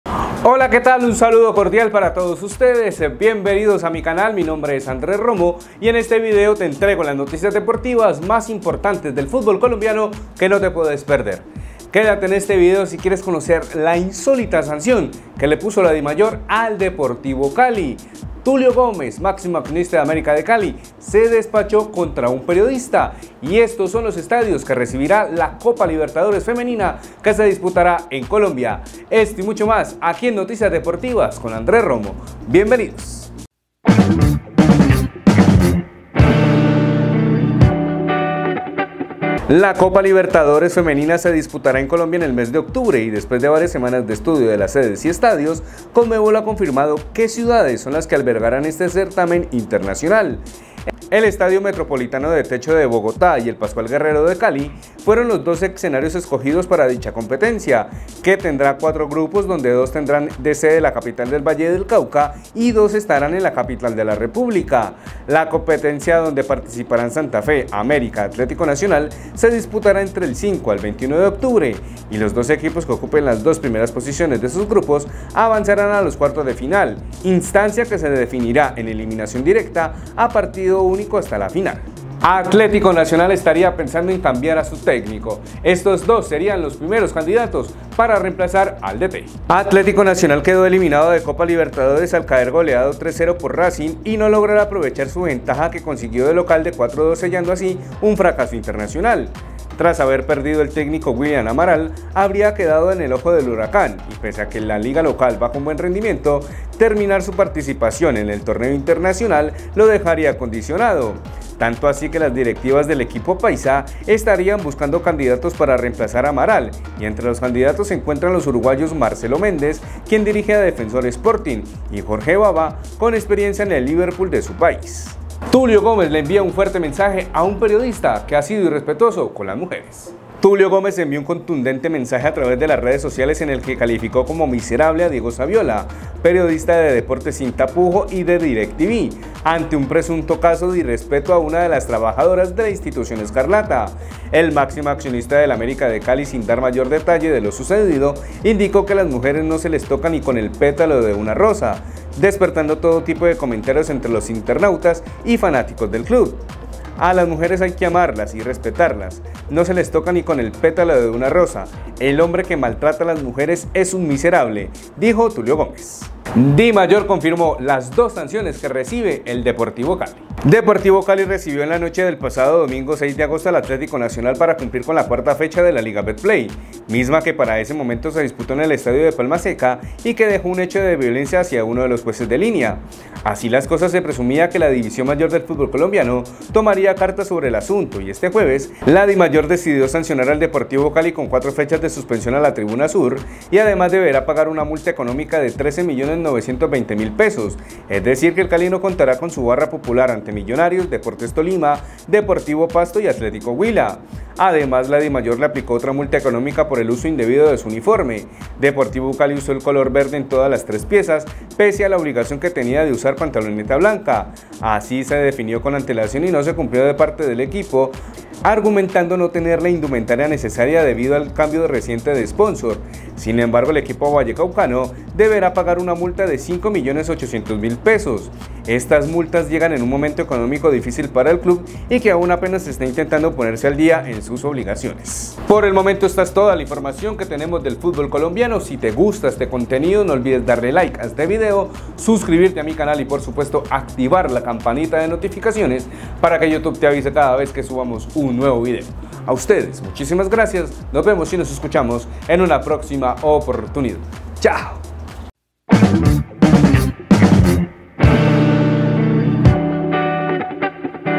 DURA SANCIÓN PARA EL CALI POR DIMAYOR ✅| NACIONAL BUSCA NUEVO DT 💣
Qué tal parceros, en este video les entrego las noticias deportivas más importantes del futbolcolombiano
- Estos son los dos estadios en colombia donde se jugará la copalibertadores femenina.
- atleticonacional estaría buscando nuevo DT y ya tiene dos candidatos.
- tuliogomez le envió fuerte indirectazo a diegosaviola periodista de cali
- dimayor sancionó doble al